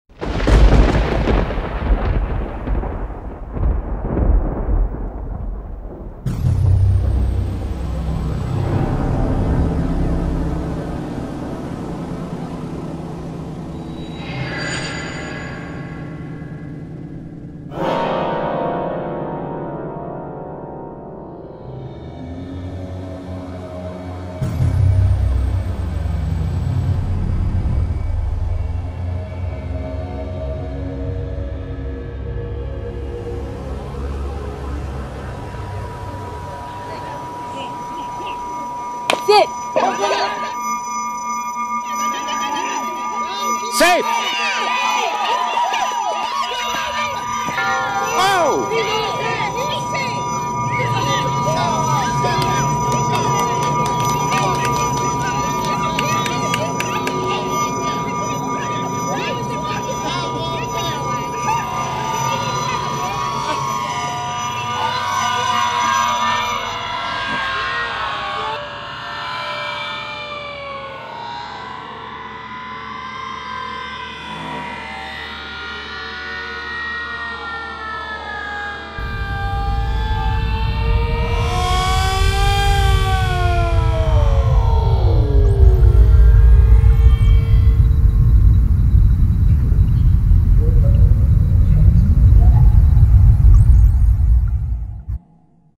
Incident At Field 2
Nicky T takes a run at home plate and gets jacked up!